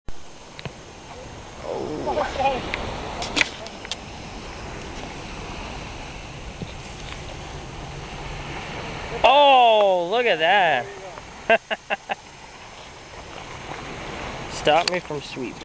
Bocce ball fun at Presque Isle in Marquette, MI
Playing "boundaryless" bocce ball.